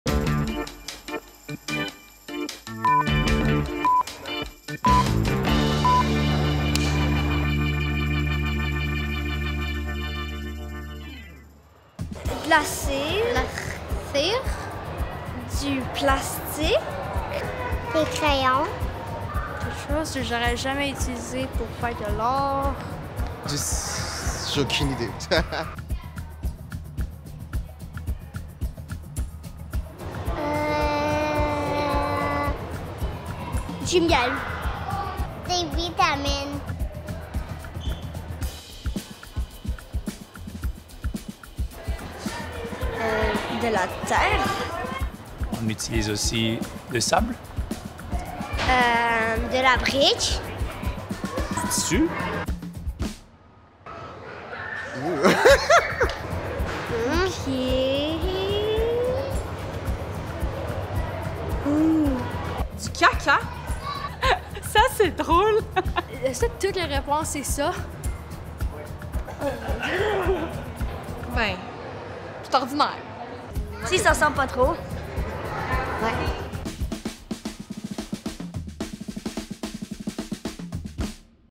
Oh merde! Repensons les déchets humains
Que savez-vous de la ressource infinie la plus incomprise au monde?

Découvrez ce que pensent les visiteurs du musée et visitez Oh Merde! Repensons les déchets humains au Musée des sciences et de la technologie du Canada, du 10 mai 2024 au 5 janvier 2025.